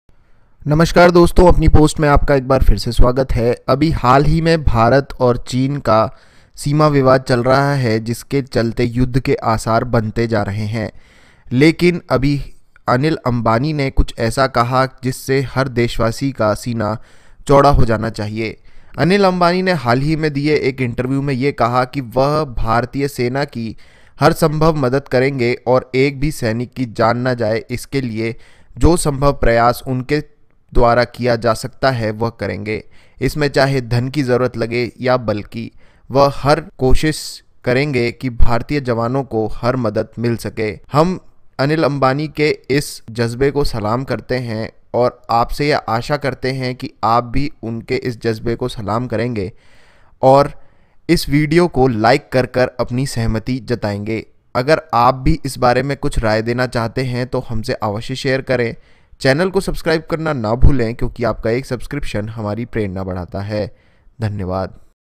अनिल अम्बानी करेंगे चीन के खिलाफ भारतीय सेना की पूरी मदद
आज हम आपको बताएँगे की अनिल अम्बानी ने चीन के लिए क्या कहा।  अगर आप सच्चे देश भक्त है तो कमेंट बॉक्स में जय हिन्द जरूर लिखें।